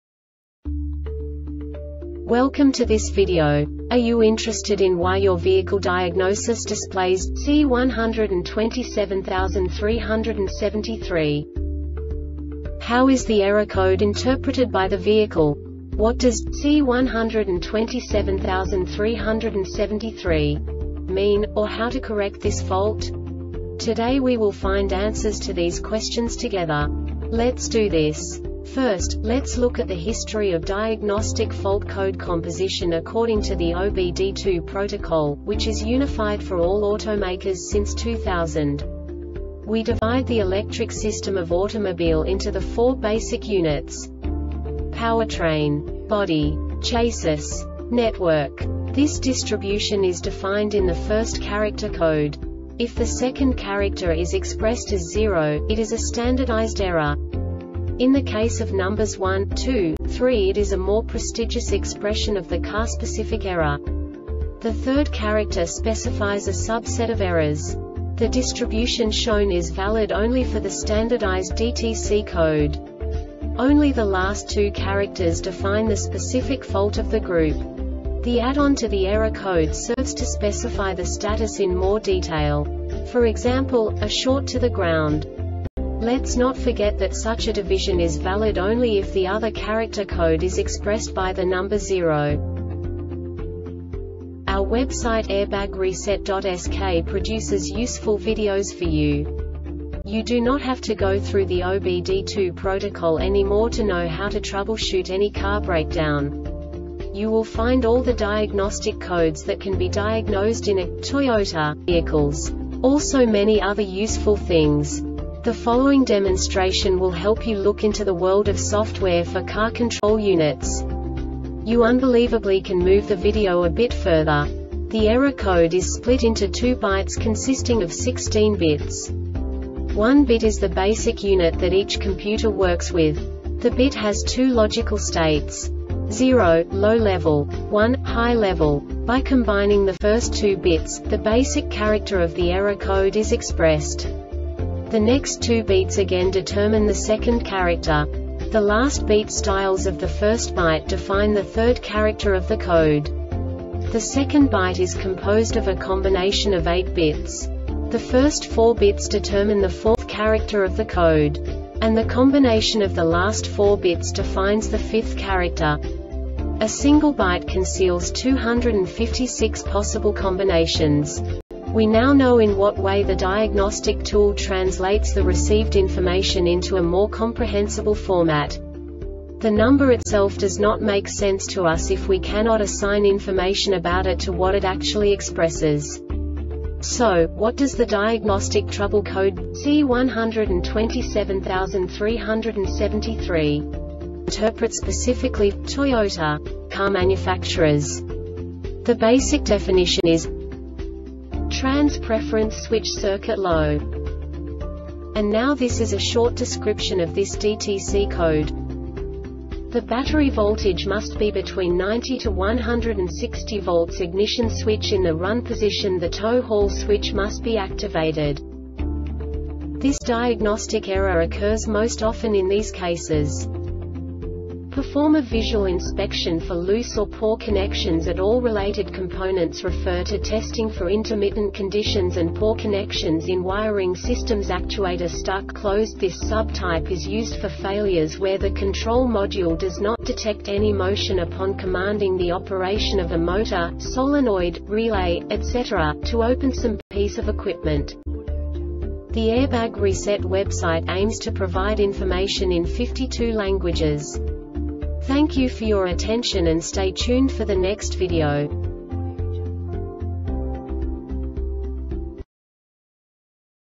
DTC Toyota C1273-73 Short Explanation
Contents:
0:21 Basic DTC analysis according to OBD2 protocol standard.
1:48 Insight into programming
2:58 A diagnostically understandable description of the Diagnostic Trouble Code
3:05 Basic error definition

Make:
Toyota
Code:
C1273 73
Definition:
Trans Preference Switch Circuit Low
Description: